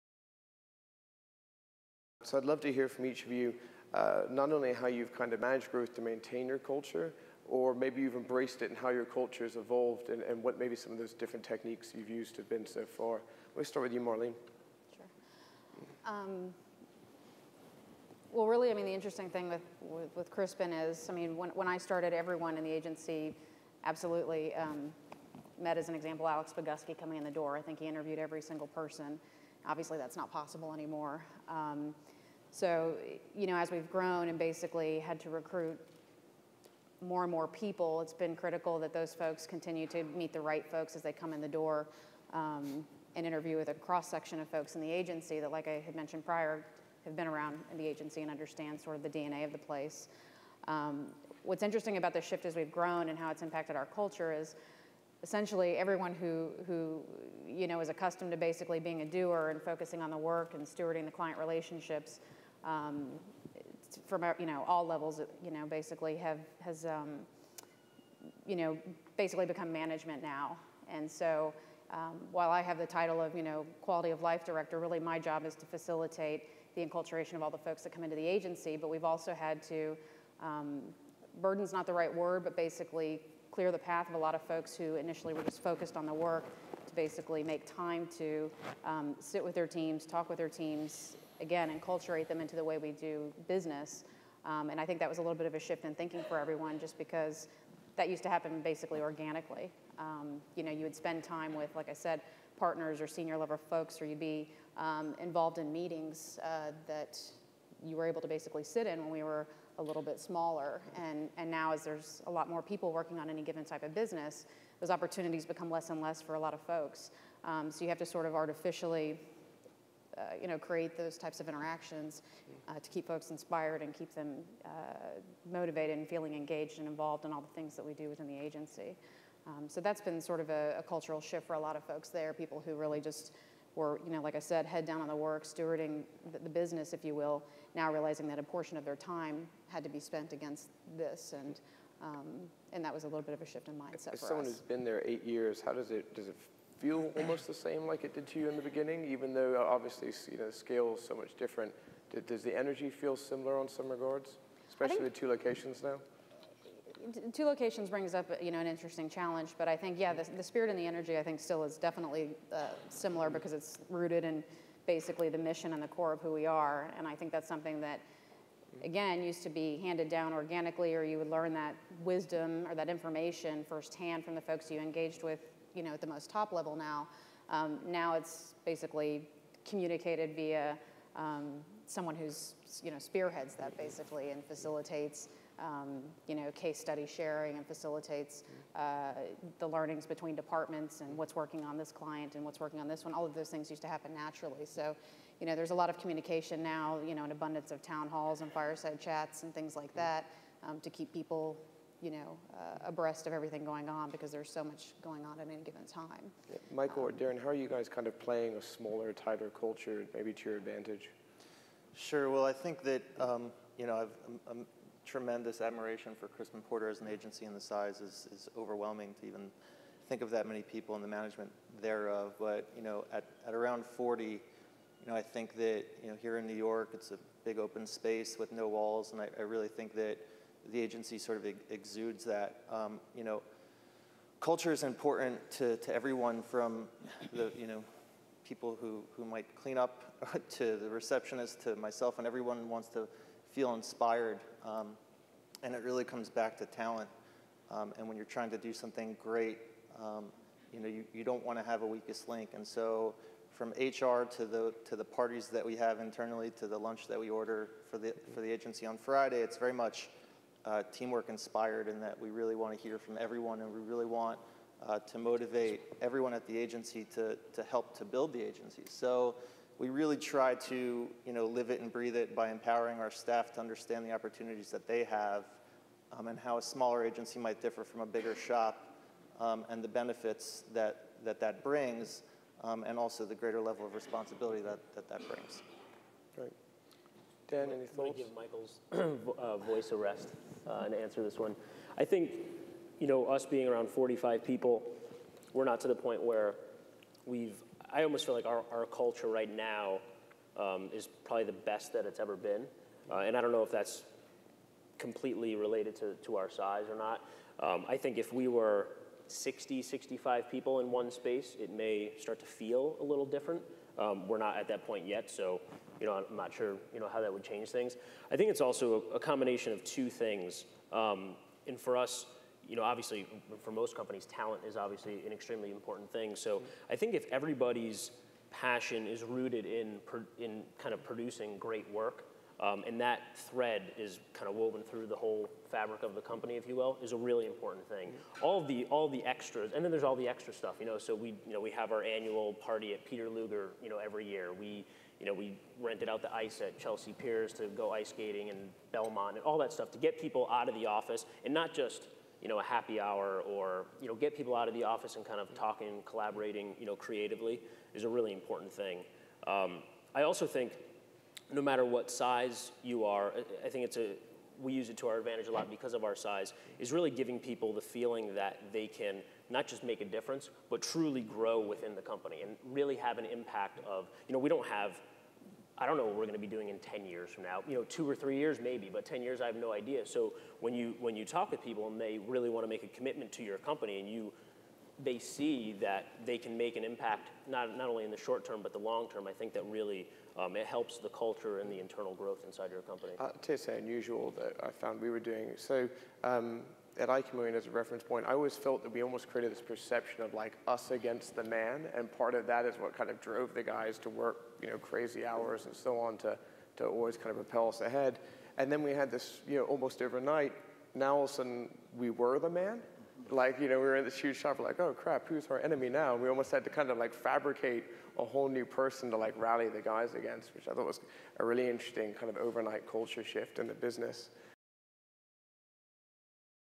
Growing up without growing wrong
Freddie Laker moderates a panel on growth at the 2008 IAB Agency Summit